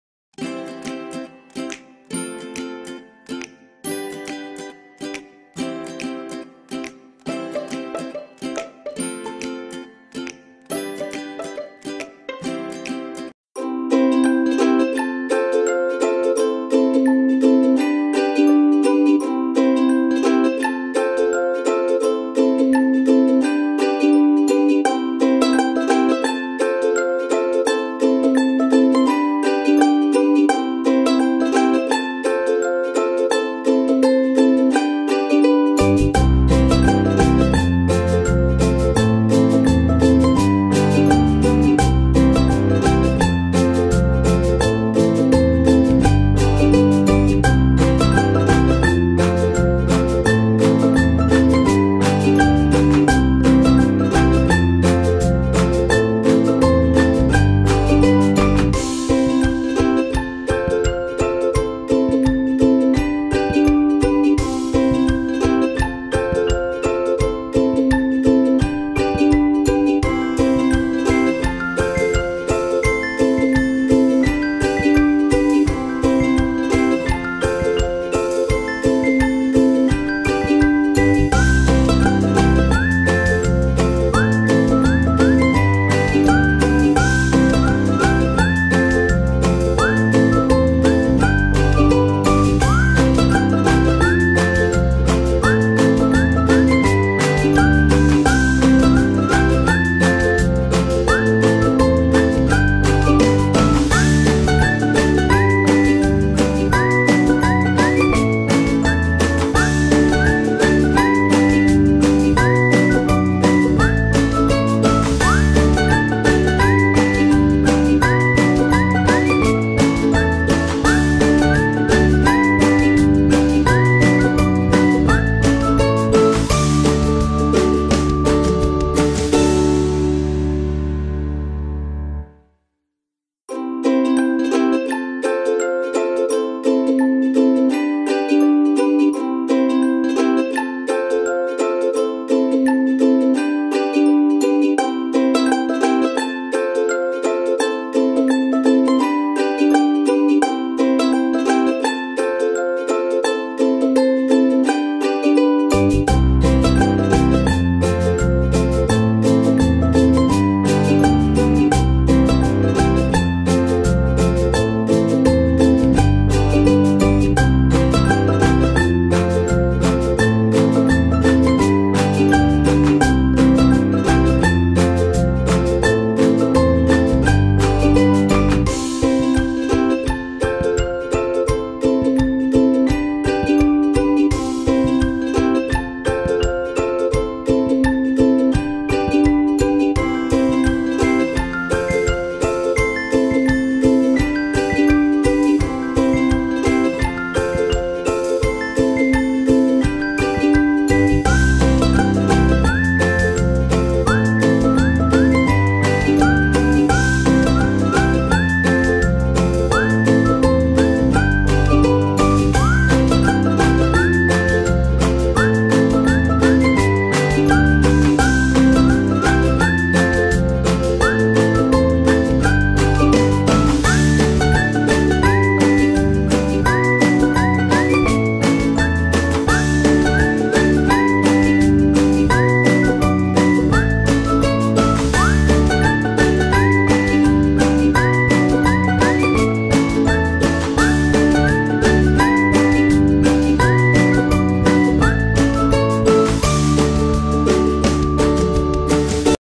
Georgette Saree WITH water zari #
VNS_Weave
This saree is most beautiful khaddi Georgett water zari saree
this saree is super Quality

BANARASI HANDLOOM PURE Georgette Saree WITH water zari

Georgette Khddi saree
💯 pure metrial

Allover pure water zari work

Pure Georgette Khddi saree

Prebook 10 to 12 days tassels 200 charge this type